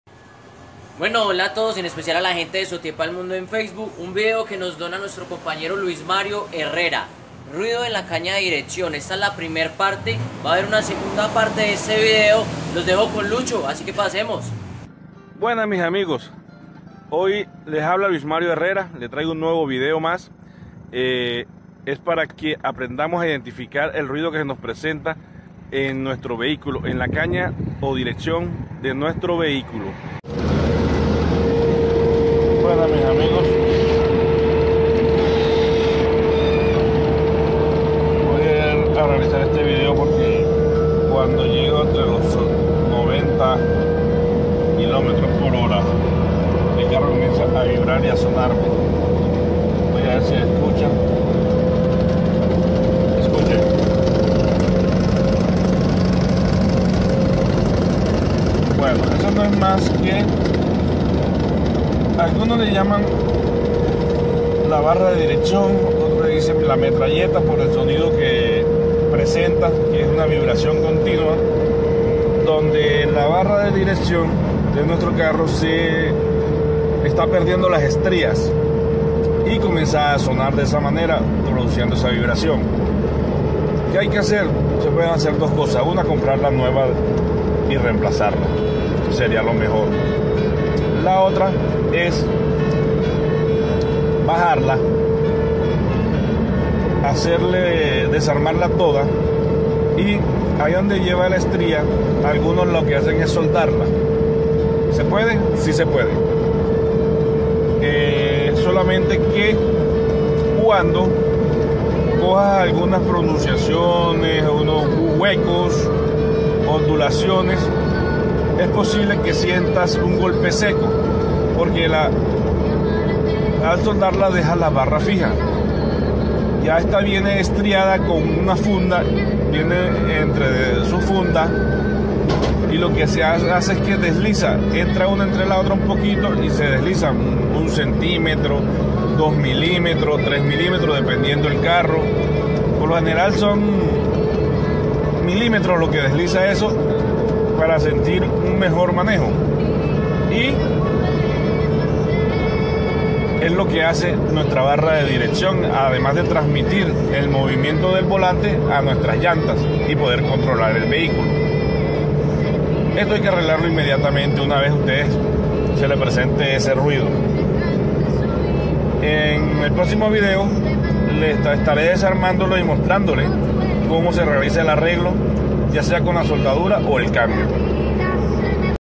ruido de la caña de dirección primera (1) parte
en este video Luis mario Herrera nos explica un poco  sobre este ruido  y en estos días nos da la reparación